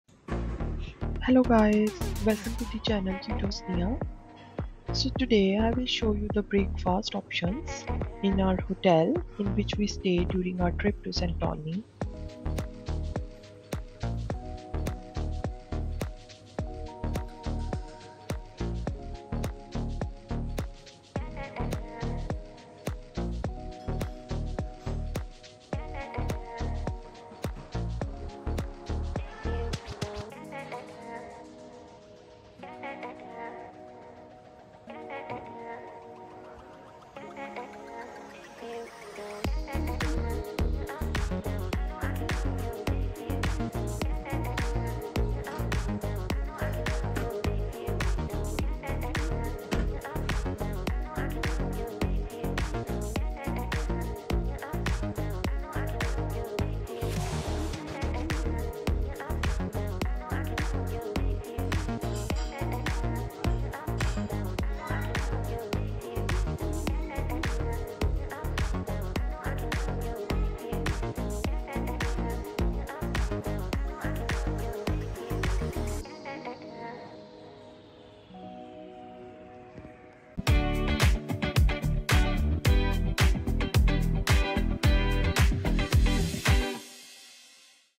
Hotel Breakfast Bar | Magma Resort Santorini | Greece Travel Vlog
Santorini Island in Greece is one of the most beautiful places in the world and here is our recommendations for Oia, Santorini

Santorini Hotel Breakfast Bar
Magma Resort by Hyatt
Greece Travel Vlog

cutosniya
cuto sniya
sniya
sniya sniya
cute sniya advenutures